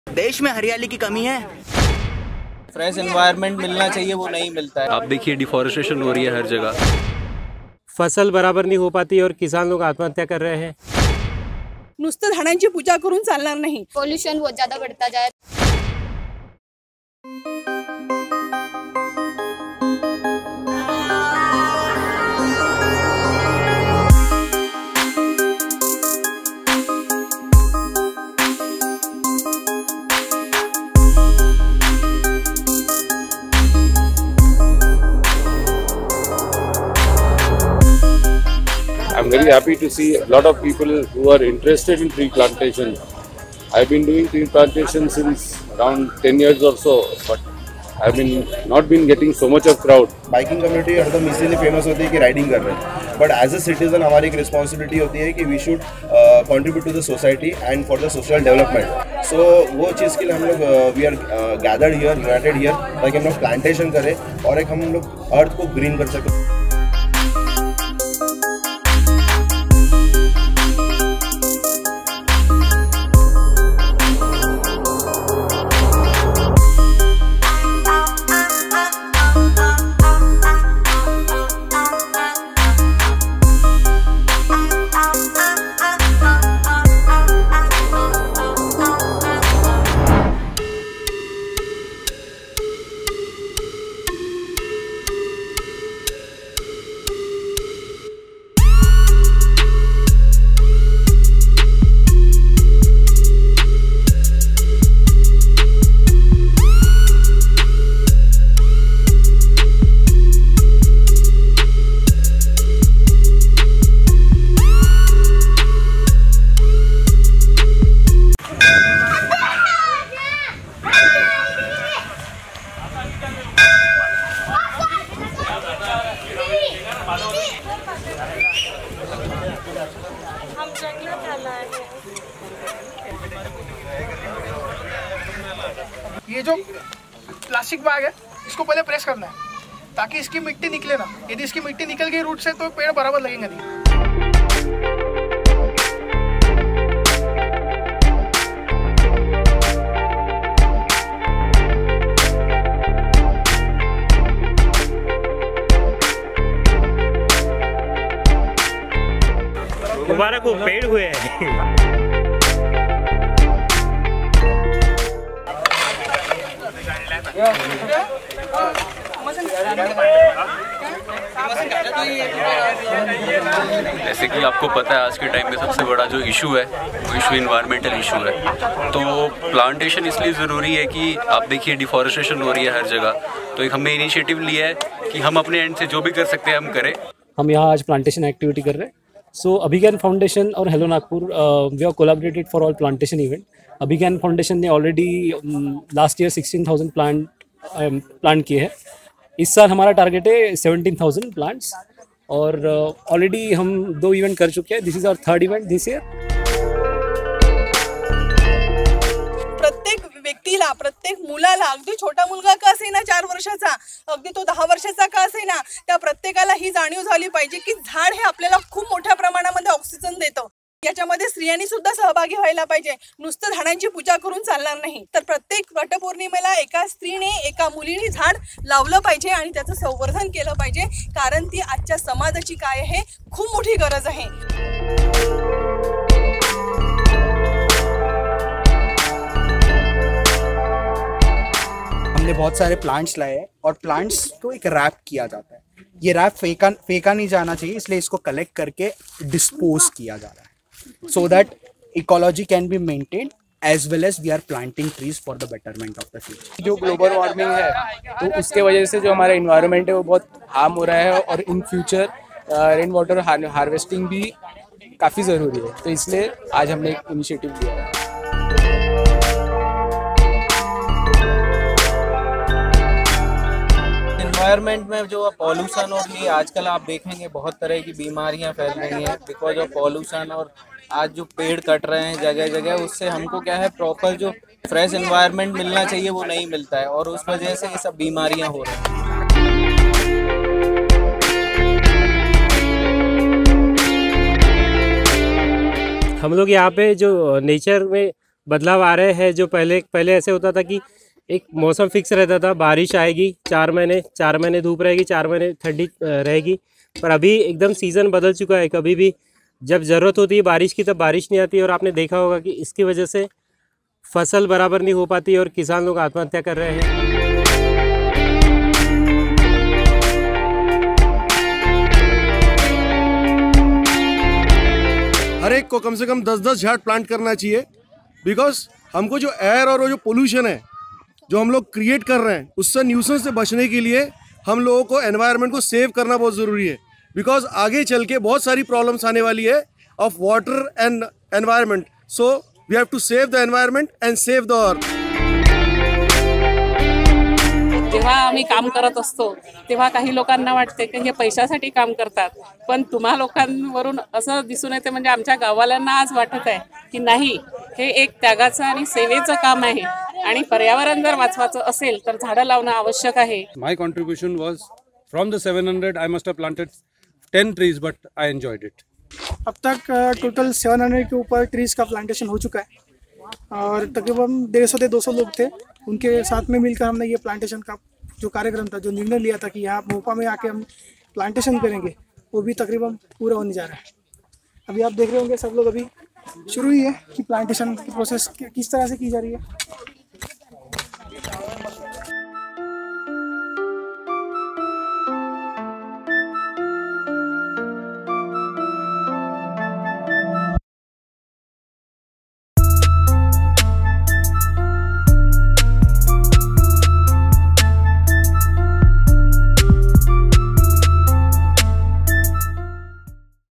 Save Earth Ride 2019
This is for the first time happening in Nagpur City. Biking Clubs and Nature Conservation Clubs came together to save earth by doing tree plantation at Mohpa. This Event was organized by Orange City Riderz and Abhigyan foundation at Nagpur City. This is dual effort of two communities to save earth and to be part of national cause.
On this same day, around 200 plus Biking clubs are doing tree plantation at 100 plus cities of India. This national Initiative was taken by Biking Community of India and powered by Apollo tyres. Around 200 people, contributed and supported this tree plantation cause at Mohpa. Around 1000 Plantation were done at Mohpa. This event would not be successful, without the joint Support of Unido Riders, Hello Nagpur, Radio Mirchi and Liqui Moly Nagpur and bikers of Nagpur.

For new opportunities, latest videos & films or updates